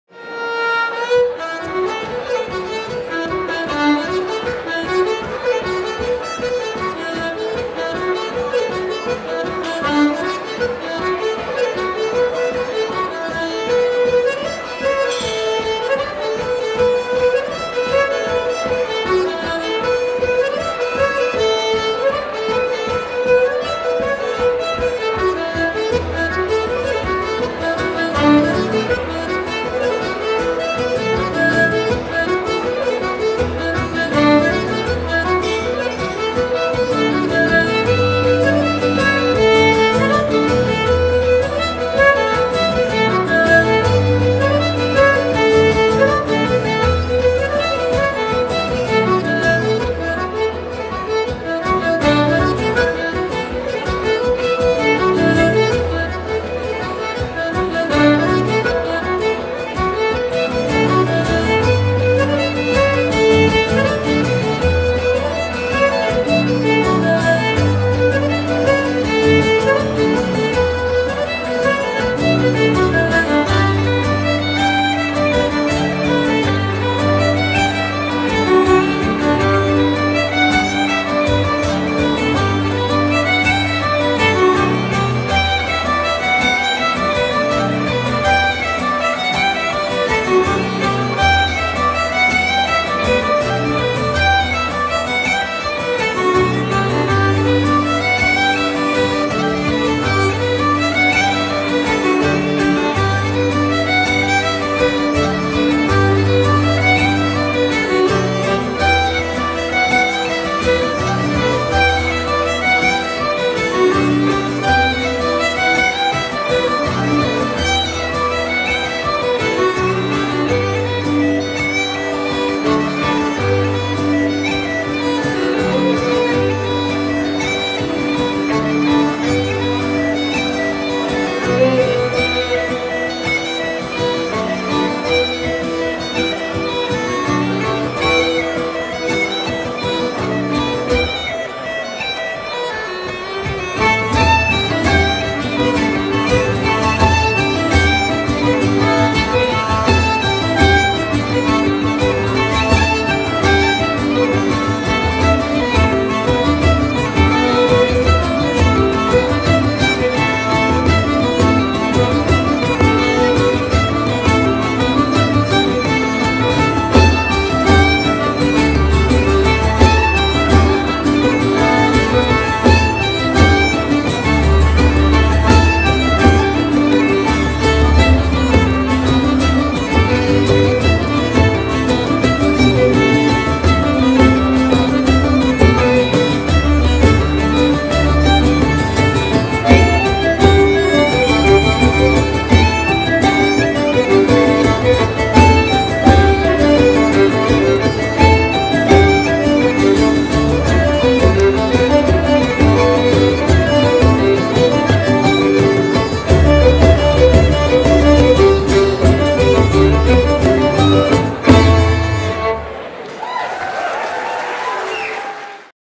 Droichead - 3 Polkas - The Irish Festival of Oulu 2017
Droichead at Uusi Seurahuone, The Irish Festival of Oulu, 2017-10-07
Samppa Saarinen - uilleann pipes, tin whistles, low whistle
Anna Luusua - fiddle, mandola, guitar, vocals
Vesku Niemelin - button accordion
Brent Ó Caiside - bodhrán, vocals
Jani Suvanto - guitar, bouzouki